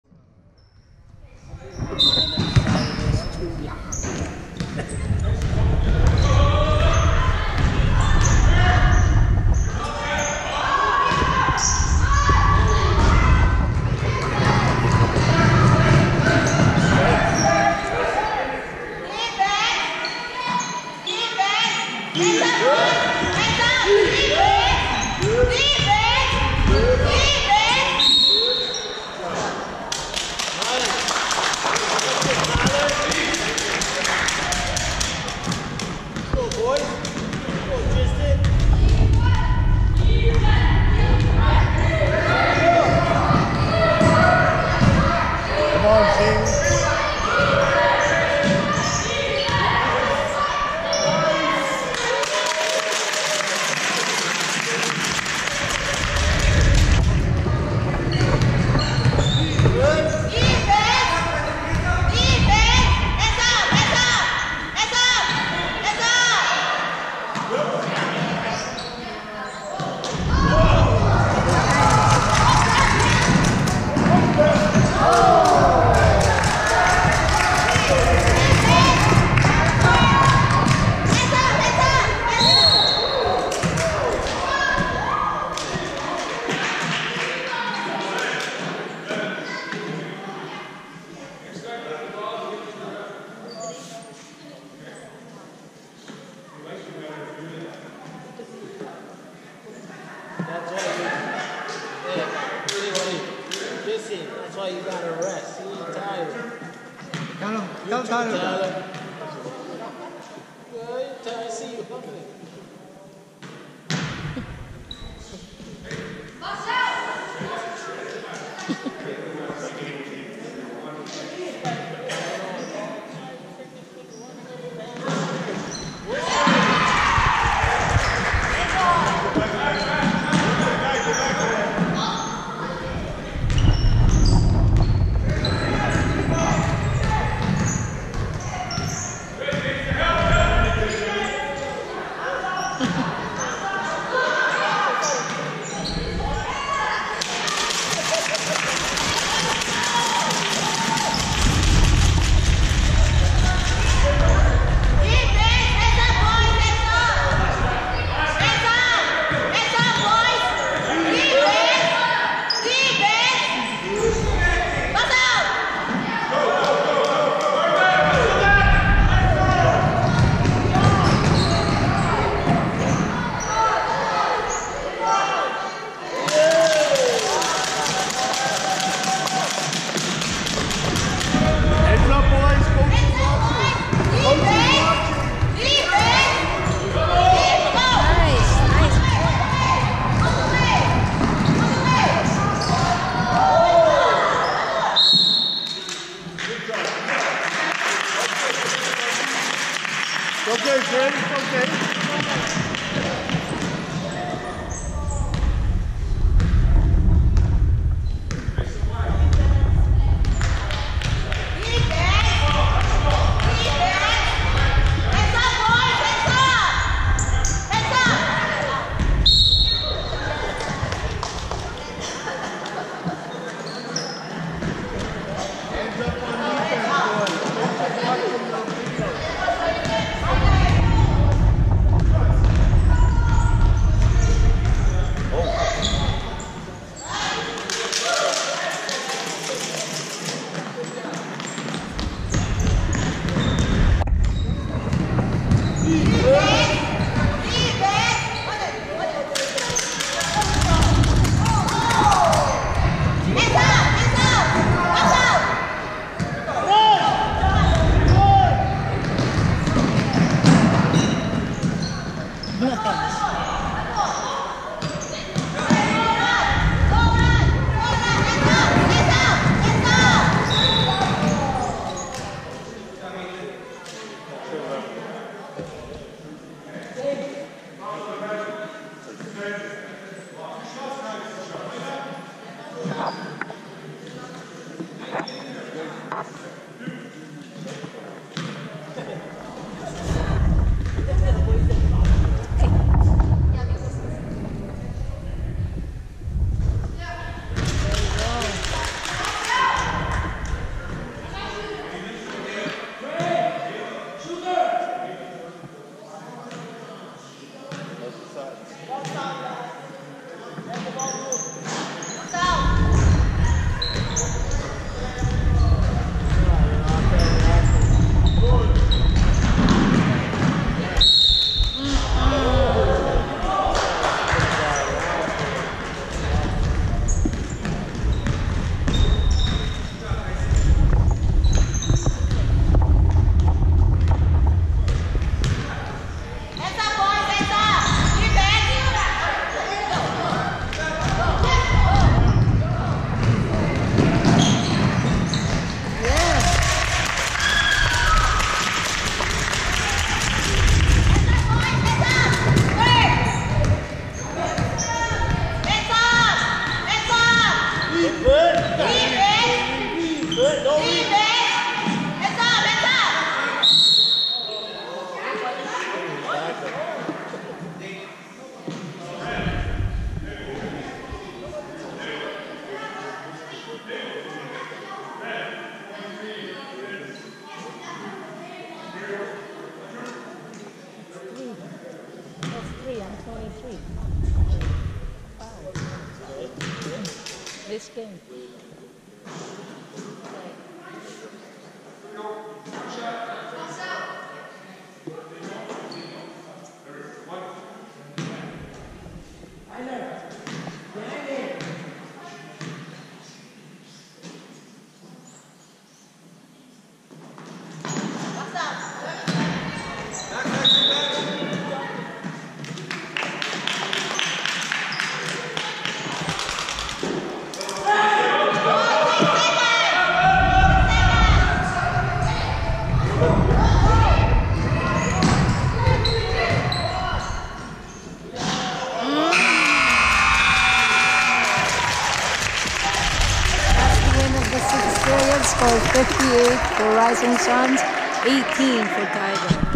DRS VS LIONS NOV.30.2019 GAME 3 P6
CHRIS PAULIN MEMORIAL TOURNAMENT, KANATA OTTAWA
U12 DRS VS CORNWALL LIONS NOV 30.2019 GAME 3
FINAL SCORE DRS 73 VS LIONS 24